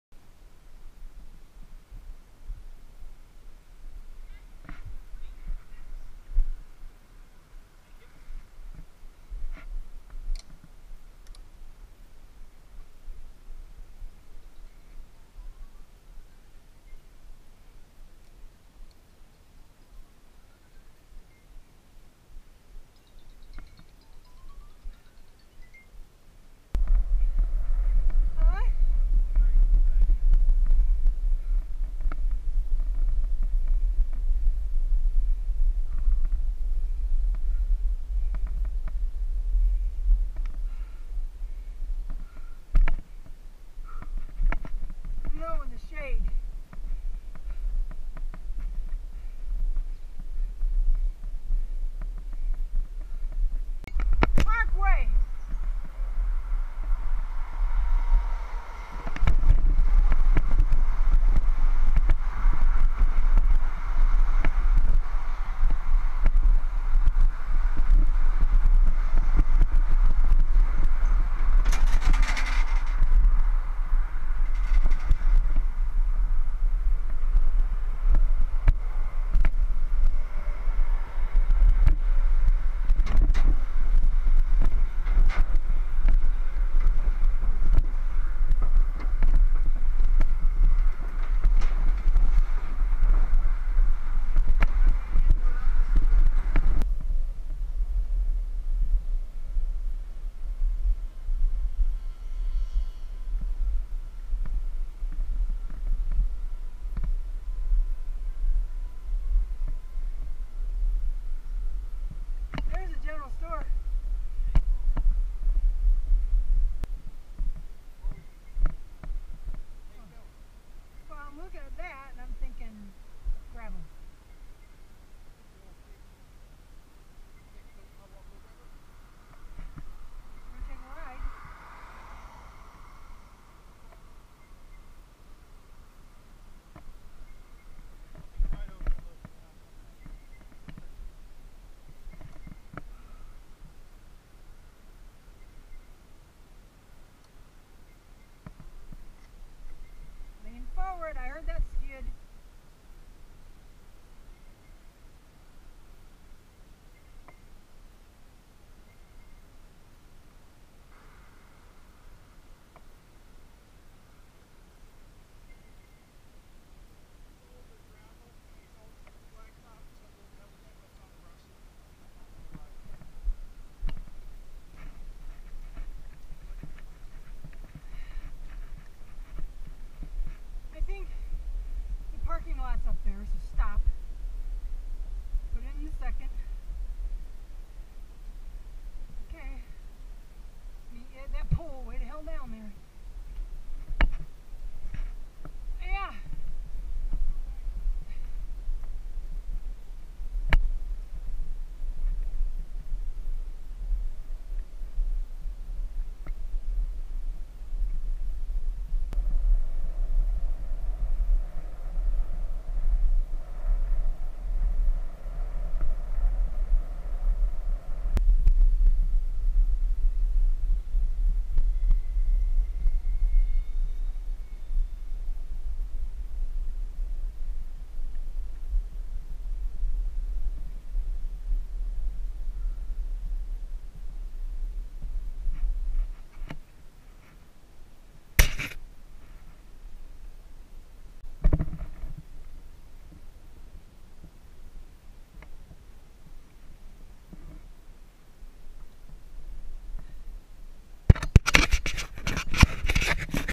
Allaire State Park Mostly Pooka
Allaire State Park New Jersey. Joe used my 36v Pon-e while I rode my T12 Roadster. We stopped a few times since it was in the heat of the day. We stopped at the local general store for a small bite of ice cream that we shared (awwww).